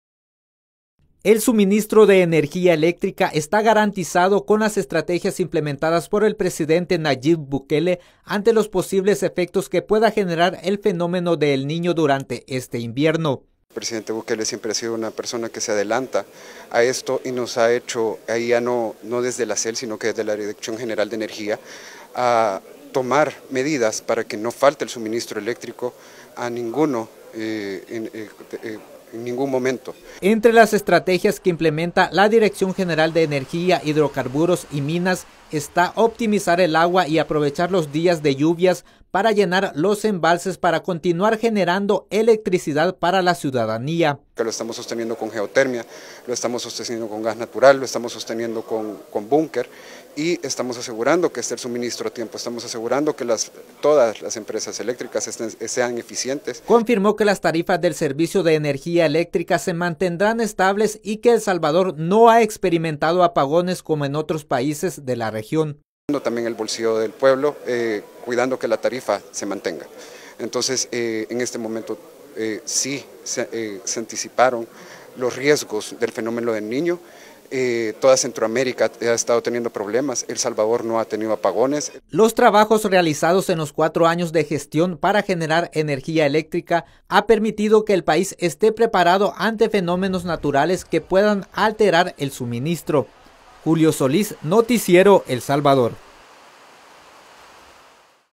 Nayib Bukele garantiza la electricidad a los hogares salvadoreños
El suministro de energía eléctrica está garantizado para la población, ante los posibles efectos que pueda generar el fenómeno del niño, debido a las estrategias implementadas por el Gobierno del Presidente Nayib Bukele.

Los trabajos realizados durante cuatro años de gestión permiten que el país esté preparado ante cualquier eventualidad.